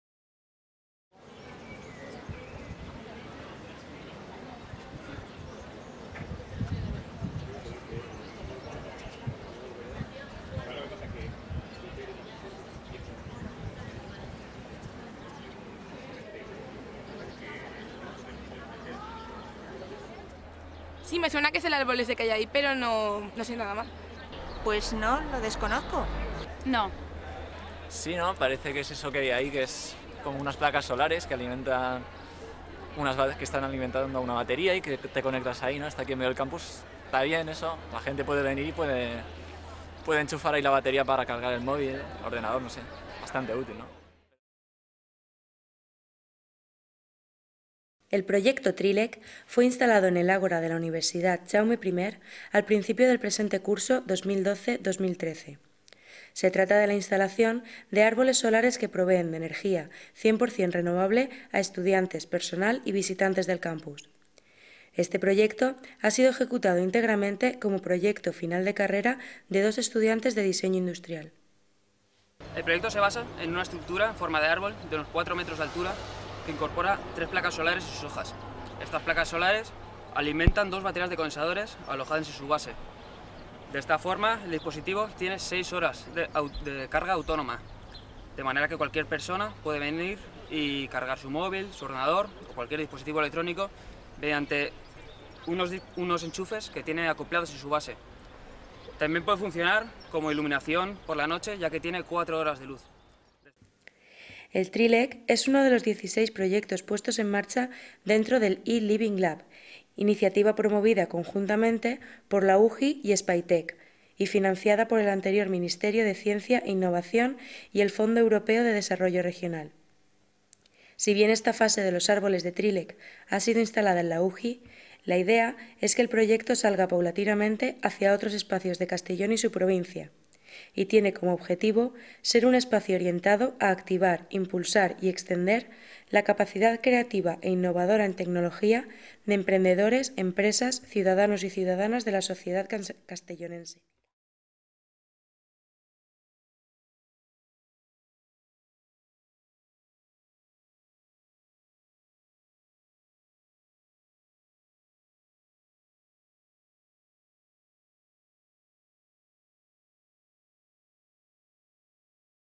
Treelec, árboles solares que proveen de energía renovable
Reportaje realizado por alumnado de Comunicación Audiovisual de la Universitat Jaume I sobre el proyecto Treelec consistente en la instalación en el campus castellonense de árboles solares que proveen energía cien por cien renovable. El proyecto se basa en estructuras de cuatro metros de altura en forma de árbol a las que, a través de la integración de varios componentes eléctricos, se les ha dotado de autosuficiencia energética, funcionando como farola por la noche y mobiliario urbano distribuidor de energía durante el día.

Vídeo producido por los alumnos del Grado en Comunicación Audiovisual de la UJI Laura Cuesta Pardo, Susana Luna Durán, Paloma Miralles Martín, Arturo Salom Pereda y Almudena Zamora León dentro del proyecto Labcom-Ciència gestionado por el profesor del Departamento de Ciencias de la Comunicación Francisco López Cantos, con la colaboración del Laboratorio de Comunicación Audiovisual y la Unidad de Cultura Científica y de la Innovación de la Universitat Jaume I.

Labcom-Ciència UJI 2013

Treelec, arbres solars que proveeixen d'energia renovable

Reportatge realitzat per alumnat de Comunicació Audiovisual de la Universitat Jaume I sobre el projecte Treelec consistent a l'instal·lació en el campus castellonenc d'arbres solars que proveeixen energia cent per cent renovable. El projecte es basa en estructures de quatre metres d'alçària en forma d'arbre a les quals, a través de la integració de diversos components elèctrics, se'ls ha dotat d'autosuficiència energètica, funcionant com a fanal a la nit i mobiliari urbà distribuïdor d'energia durant el dia.

Vídeo produït pels alumnes del Grau en Comunicació Audiovisual de l'UJI Laura Cuesta Pardo, Susana Luna Durán, Paloma Miralles Martín, Arturo Salom Pereda i Almudena Zamora León dins del projecte Labcom-Ciència gestionat pel professor del Departament de Ciències de la Comunicació Francisco López Cantos, amb la col·laboració del Laboratori de Comunicació Audiovisual i la Unitat de Cultura Científica i de la Innovació de la Universitat Jaume I.

Labcom-Ciència UJI 2013
Treelec, solar trees that provides renewable energy
EN: A report carried out by students of the Bachelor's Degree in Audiovisual Communication at the Universitat Jaume I on Treelec. This project consists in the installation of solar trees in the campus of Castelló that provide one hundred percent renewable energy. The project is based on structures of a four meters high tree, which have been equipped with energy self-sufficiency, through the integration of several electrical components, working as lamppost at night and energy supplier and urban furniture during the day.
Video produced by students of the Bachelor's Degree in Audiovisual Communication at the UJI Laura Cuesta Pardo, Susana Luna Durán, Paloma Miralles Martín, Arturo Salom Pereda and Almudena Zamora León within the Labcom-Science project managed by Francisco López Cantos, lecturer at the Department of Communication Sciences, with the collaboration of the Communication Sciences Laboratory and the Scientific Culture and Innovation Unit at the Universitat Jaume I
Labcom-Ciència UJI 2013